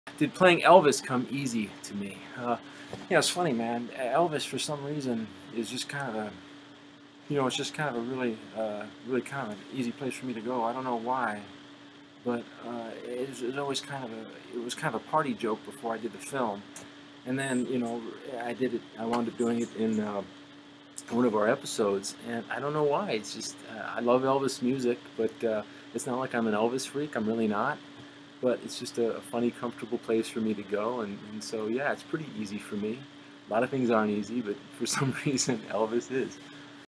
Q&A with F.B.Eye: Rick Peters part 5 - Sue Thomas: F.B.Eye
Elvis is in the building! Rick Peter aka Bobby Manning answers your question about playing Elvis in an episode of Sue Thomas: F.B.Eye. Get that episode and all the other episodes on the official DVDs or Amazon.